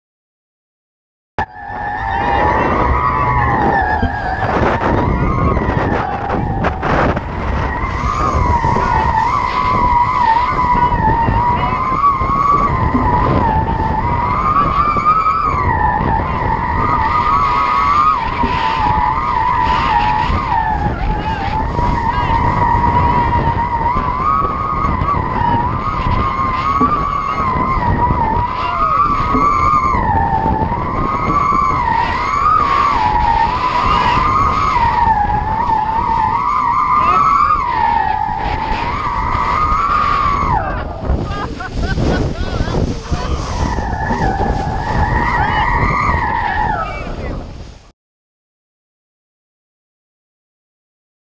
Tandem kiteboarding
Riding tandem on crazy fly tandem board 5.5mile bay crossing Corpus Christi TX.  Kiteboarding.com
Video using Beastmount and session4 GoPro